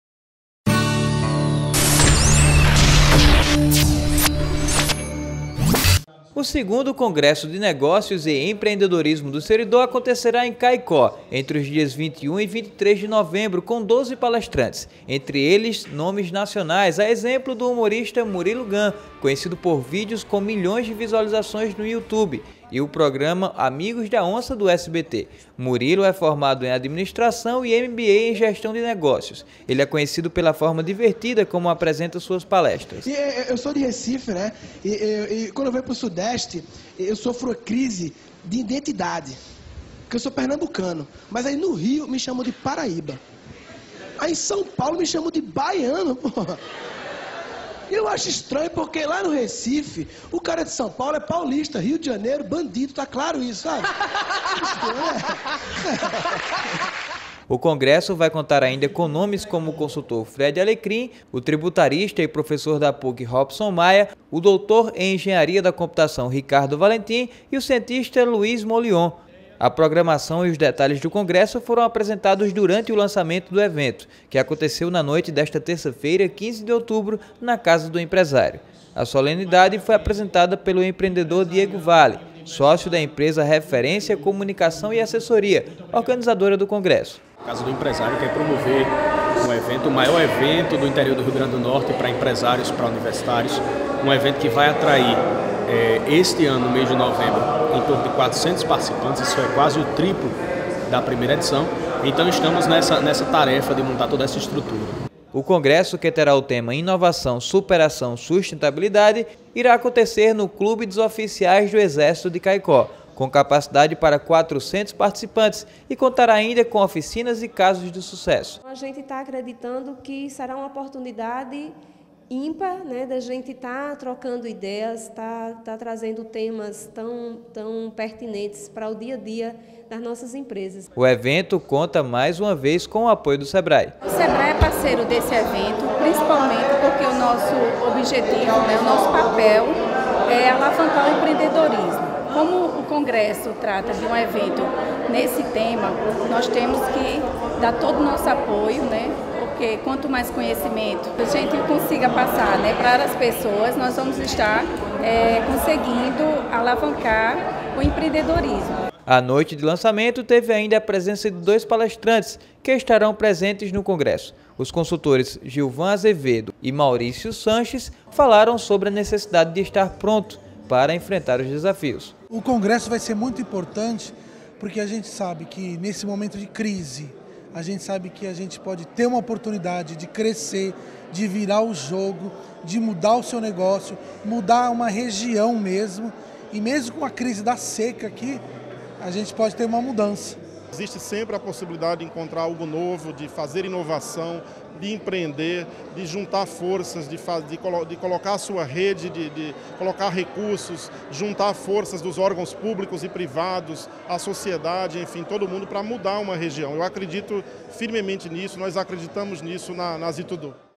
Palestrantes nacionais estarão em Caicó durante congresso de empreendedorismo
O evento vai acontecer entre os dias 21 e 23 de novembro de 2013 e entre os grandes nomes, estará o humorista Murilo Gun.